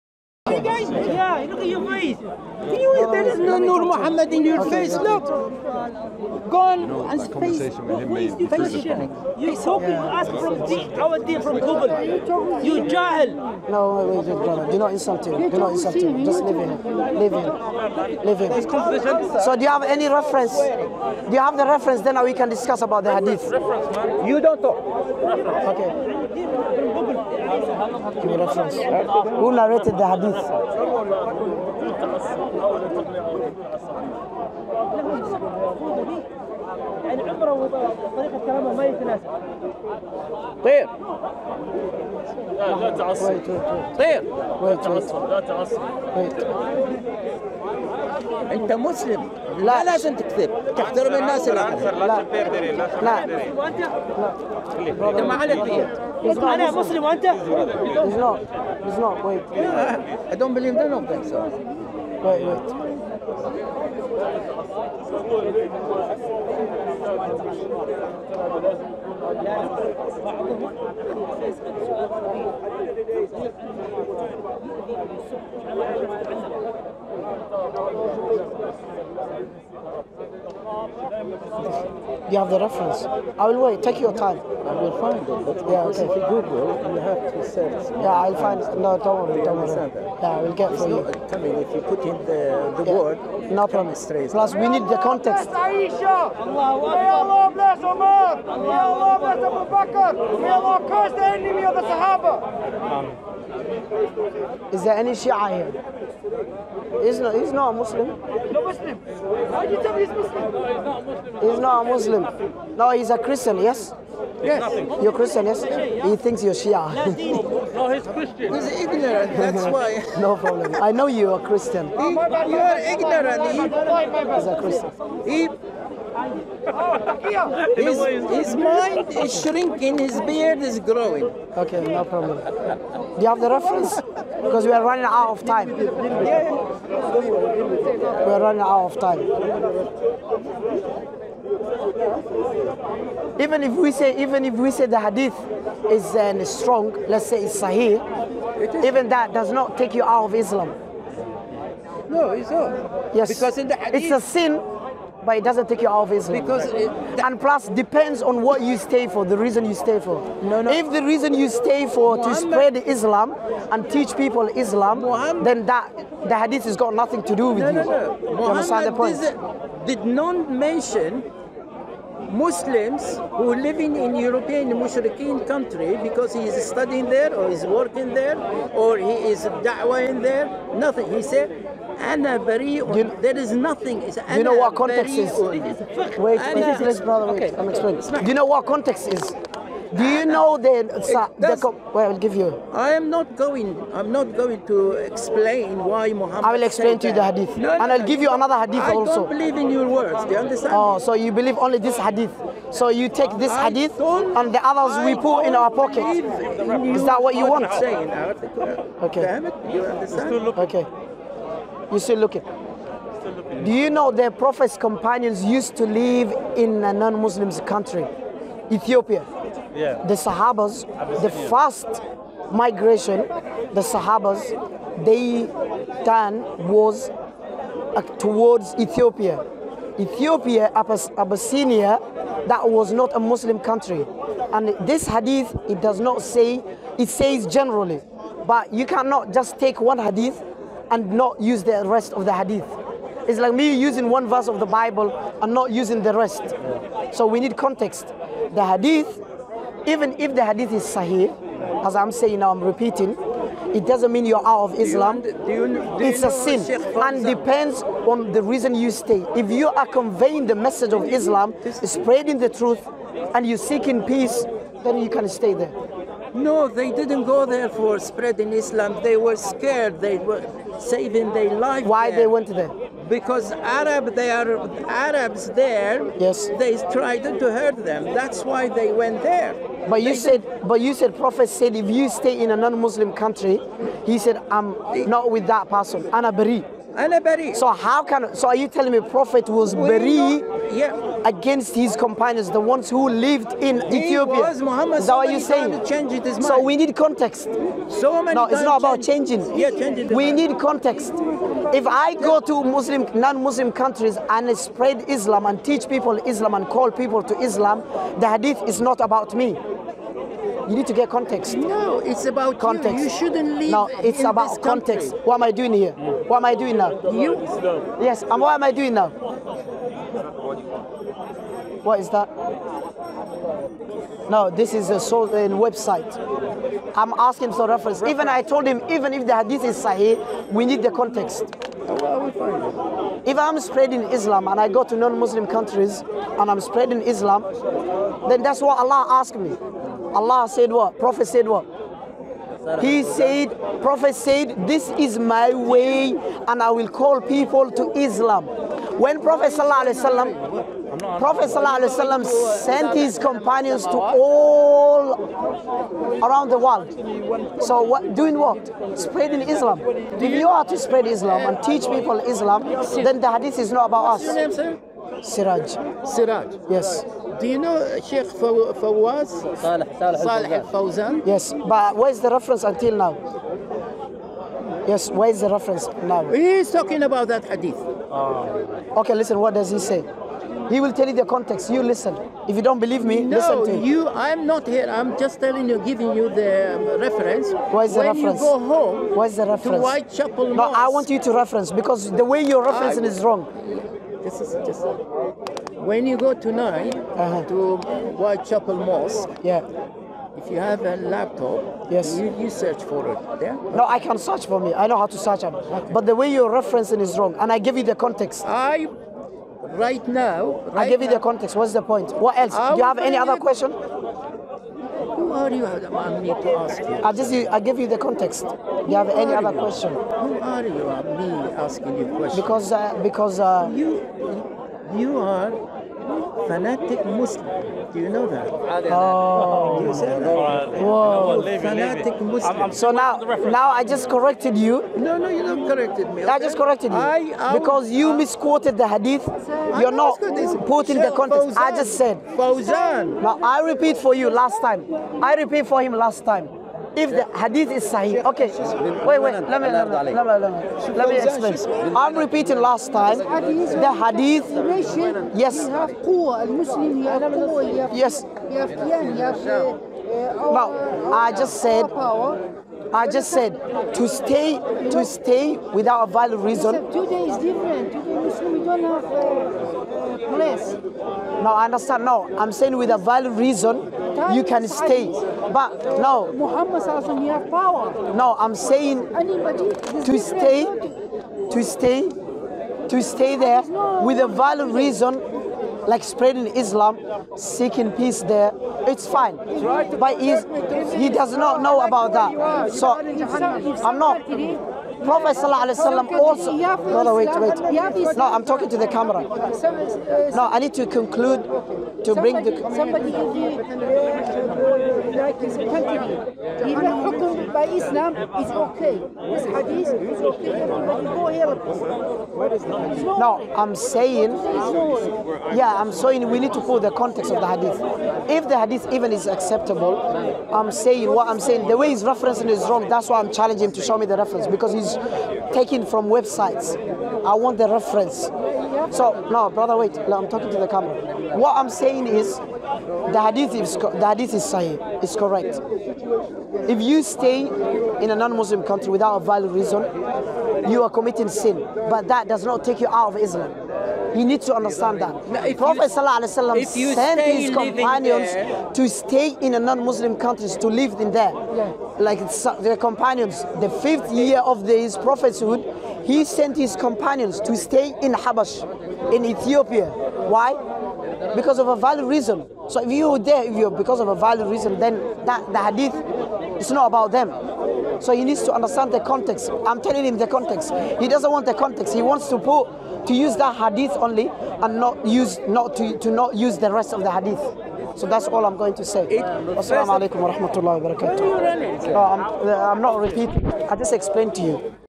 Claimed You Can't Live In Non-Muslim Country ! Siraaj VS Very Confused Creature | Speakers Corner
Confused Creature with his claim
speakerscorner in Hyde park